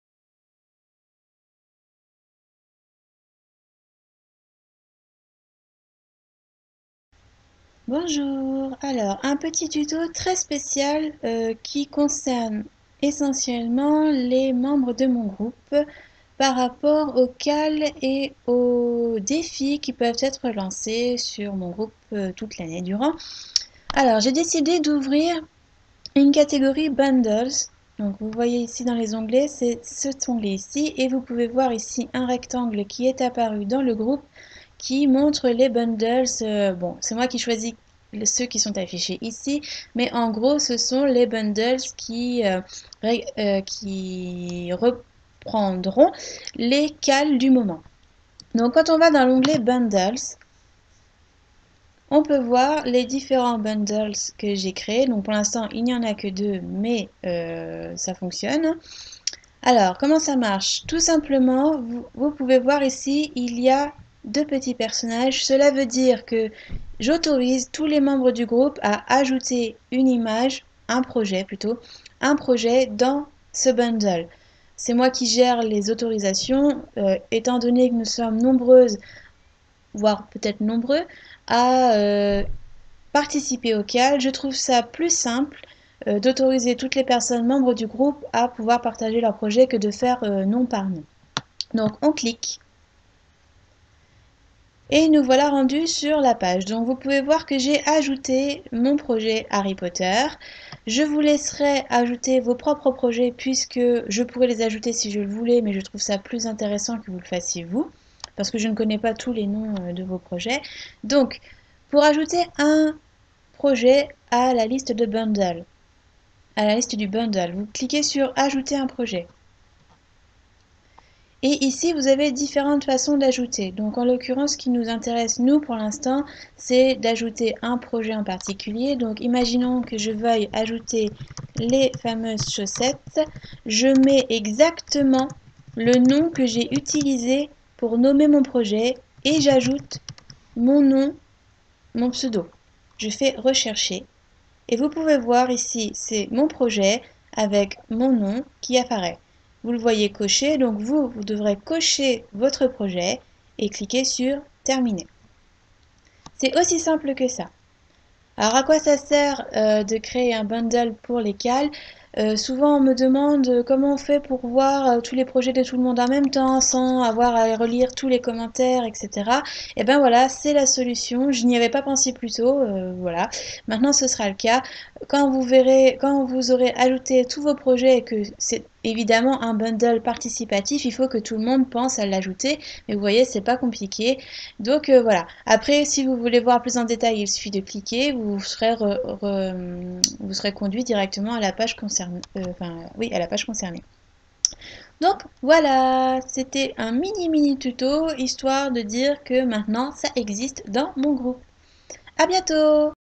Vidéo spéciale : ajouter une photo aux bundles de mon groupe (KAL et défis)
Une petite vidéo pour celles et ceux qui ne connaîtraient pas la fonction bundle dans les groupes Ravelry ;)